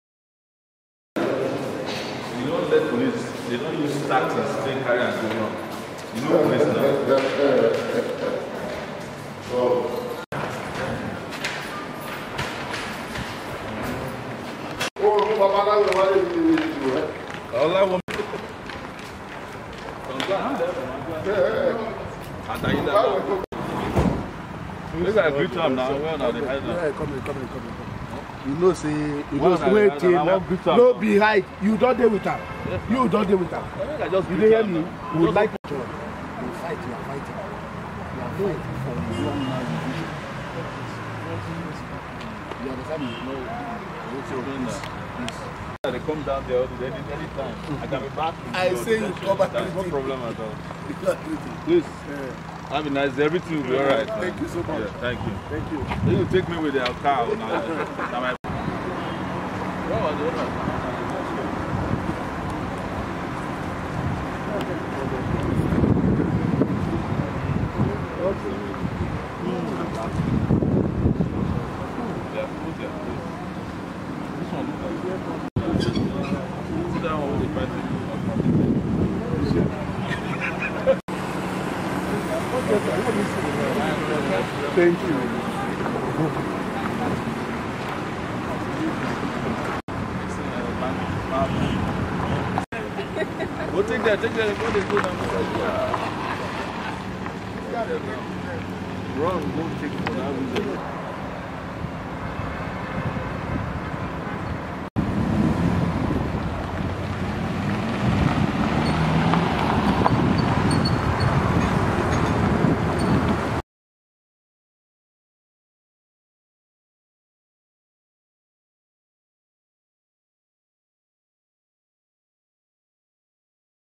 Sowore Storms Court In Support Of Pharmacist Detained For Over 1yr Over Twitter Post Against Buhari
Sowore Storms Abuja Court In Solidarity With Pharmacist, Solomon Akuma, Detained For Over One Year Over Social Media Post against Against President Buhari